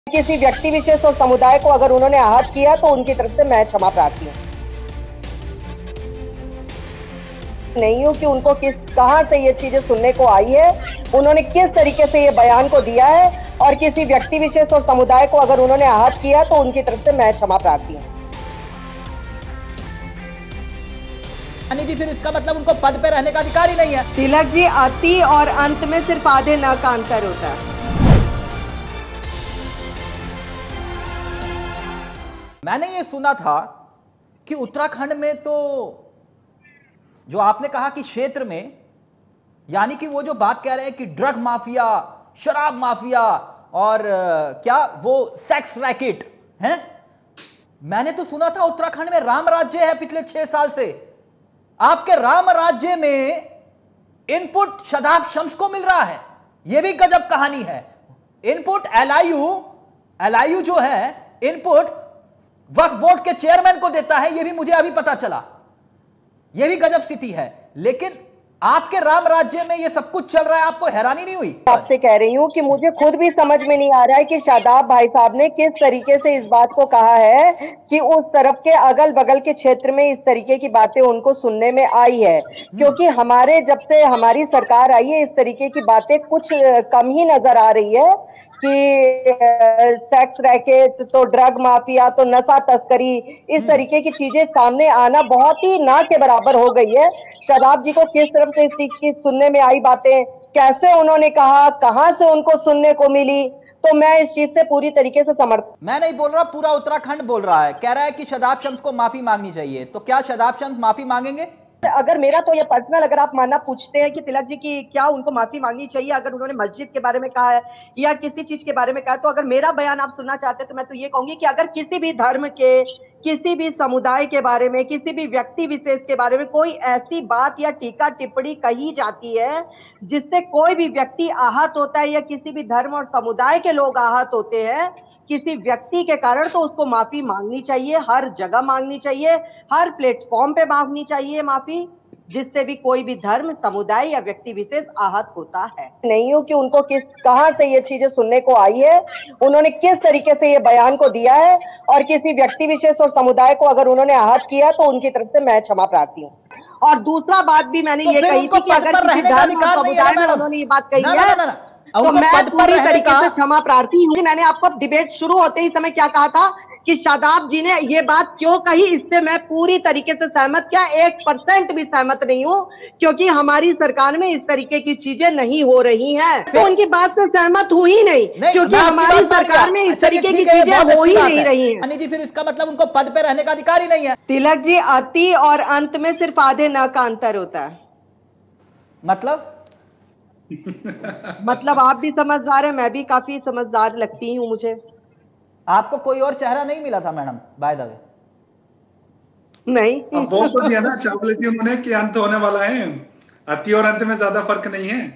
Live debate में बीजेपी नेता ने मांगी माफी । पिरानकलियर को लेकर दिए शादाब शम्स के बयान को लेकर बवाल
देवभूमि उत्तराखंड में पिरान कलियर एक ऐसा स्थान है जो मुस्लिम समुदाय के साथ-साथ हिंदू आस्था का भी प्रतीक है। राजनीतिक दलों के नेताओं के तमाम बड़े नामों के बीच प्रधानमंत्री नरेंद्र मोदी का बड़ा नाम भी आता है जिन्होंने श्रद्धा भावना के तहत पिरान कलियर में अपने नाम की चादर चढ़वाई है। भाजपा नेता और वक्फ बोर्ड के चेयरमैन शादाब शम्स ने पिरान कलियर पर एक विवादित बयान देकर एक नया विवाद खड़ा कर दिया है। शादाब शम्स के इस बयान के बाद राजनीतिक दलो ने उन पर चौतरफा हमला बोल दिया है। वहीं bjp भी अब इस बयान के बाद डैमेज कंट्रोल में जुटी है और शादाब शम्स को बचाने की पूरी कवायद कर रही है।